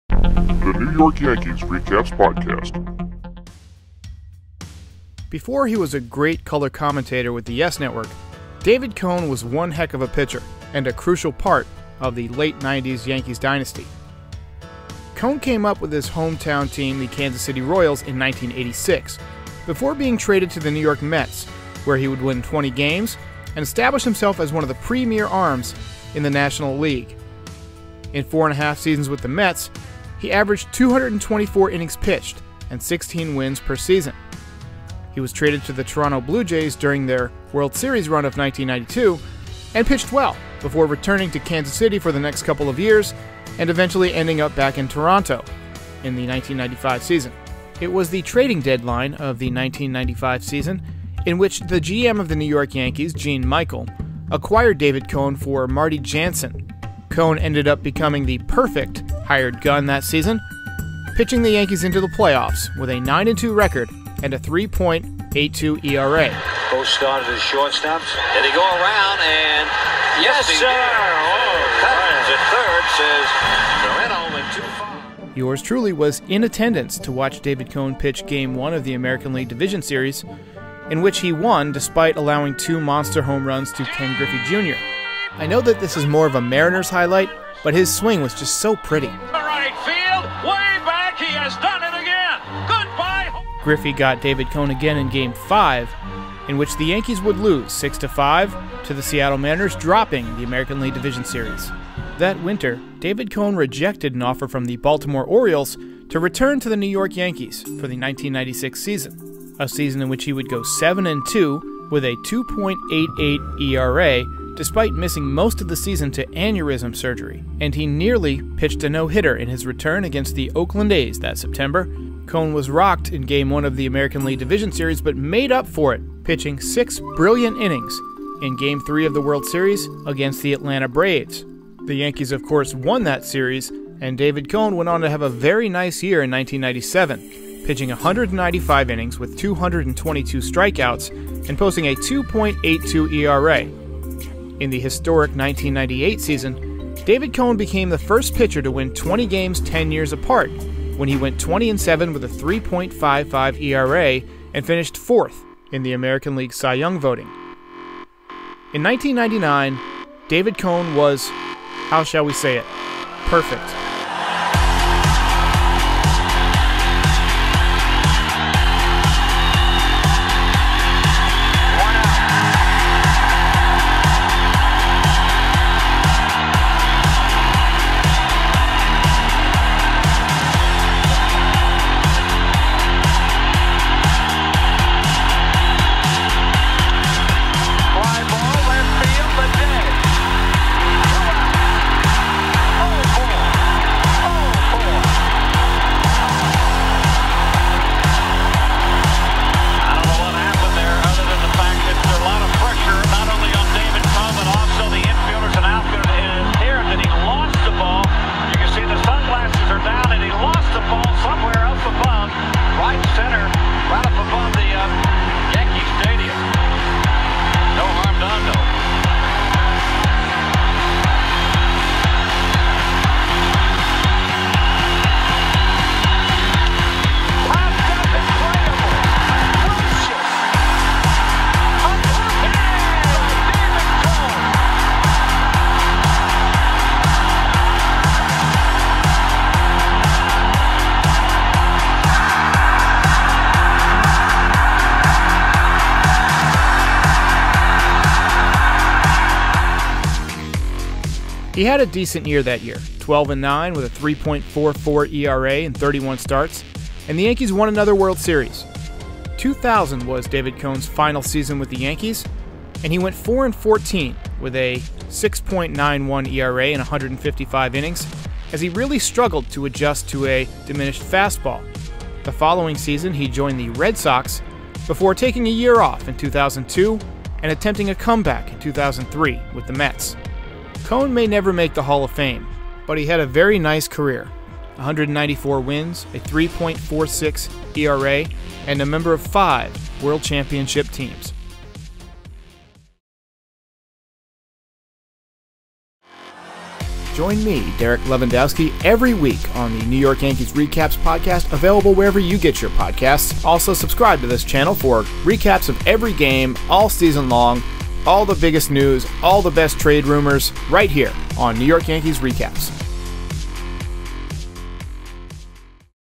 The Pefect Hired Gun
A look back at the career of David Cone, who was a member of the New York Yankees from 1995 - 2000.